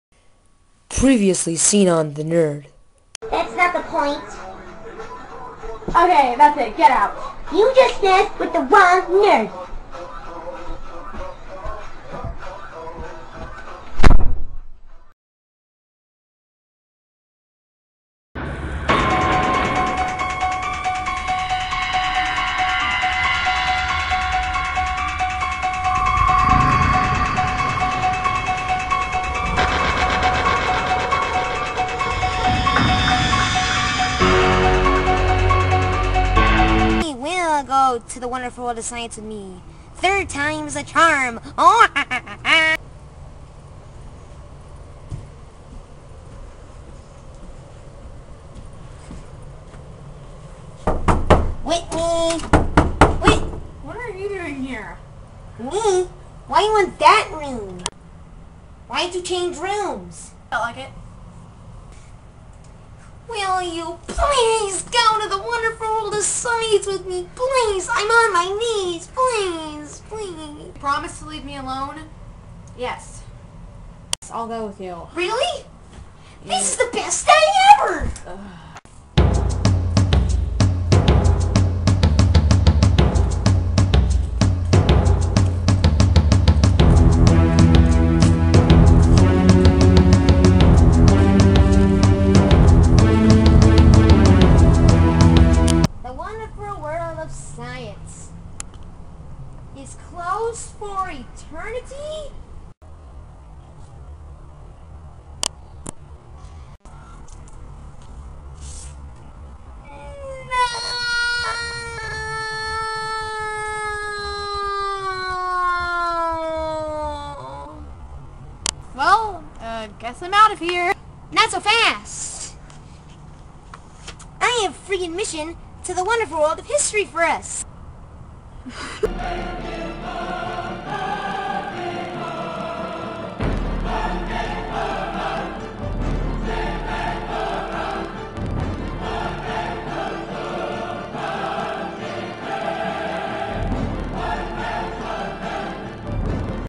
The Nerd Is Back!
Many thought the nerd would never return... but they were wrong! The Nerd is Back! And he's hoping that Whitney will go to the Wonderful World Of Science with him!!